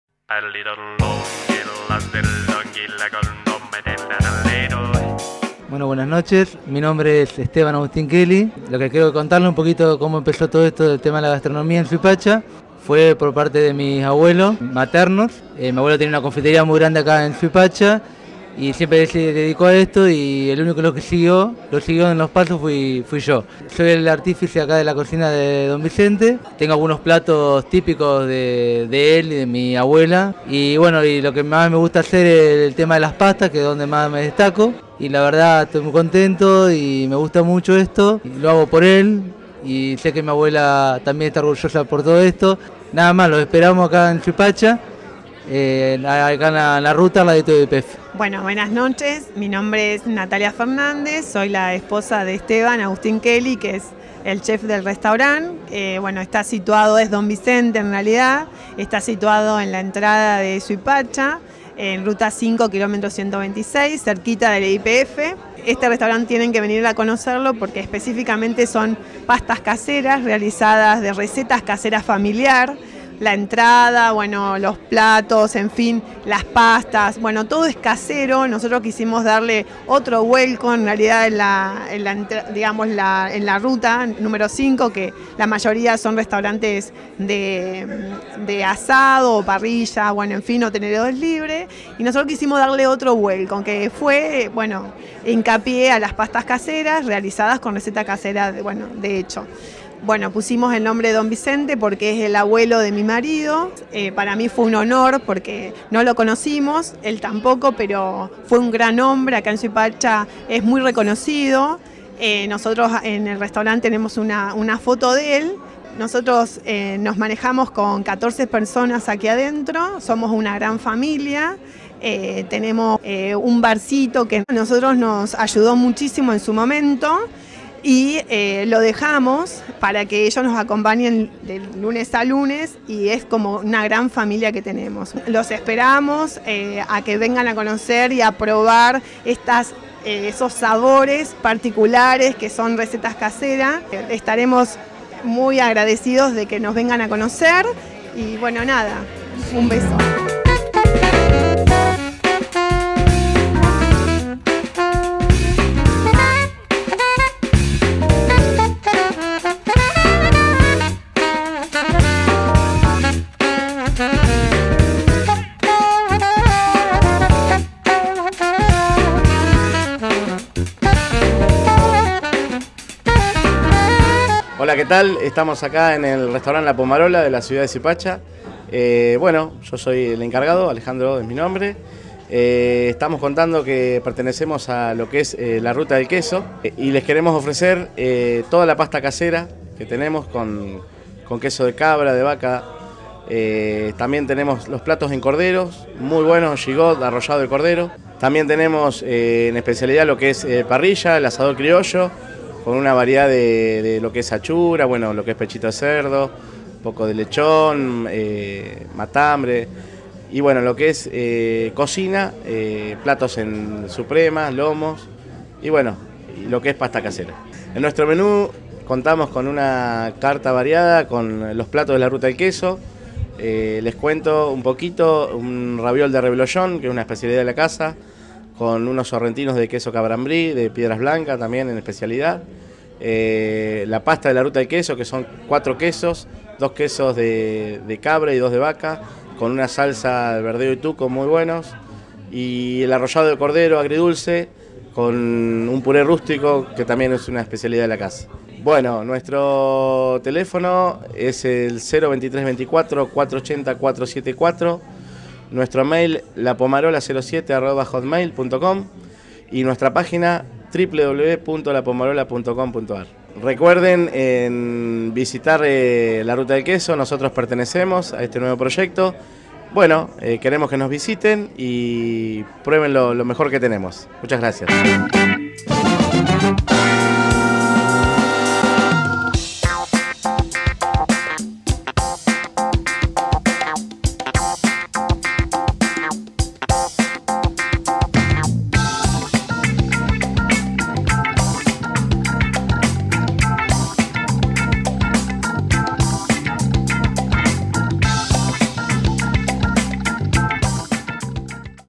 RUTA DEL QUESO Don VICENTE y POMAROLA - Suipacha
Sabores, su guia gourmet Notas Restaurant Don Vicente y La Pomarola Suipacha Pcia. Bs. As.